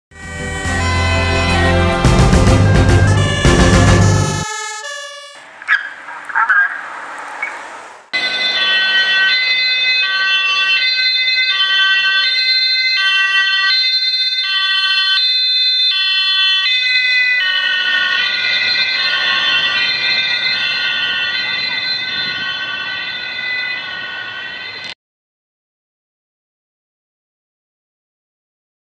sololance 07-201 vanaf post arnhem naar ongeval in Arnhem
weer een ander ongeval.